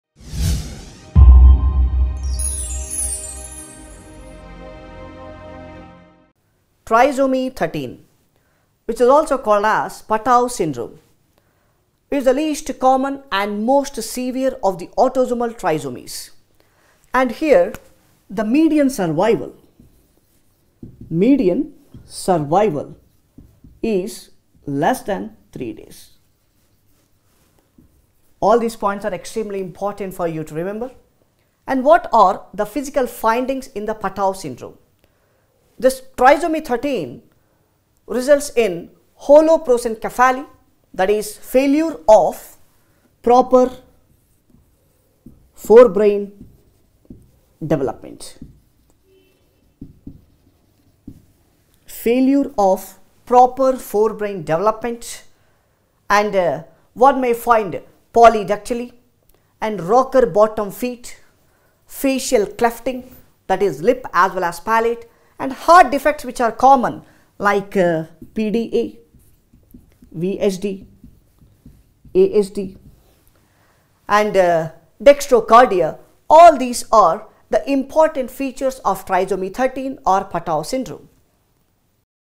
Patau syndrome - Trisomy 13 ( Usmle quick review )
This can occur either because each cell contains a full extra copy of chromosome 13 (a disorder known as trisomy 13 or trisomy D), or because each cell contains an extra partial copy of the chromosome (i.e., Robertsonian translocation) or because of mosaic Patau syndrome.

 usmle usmle step 1 uworld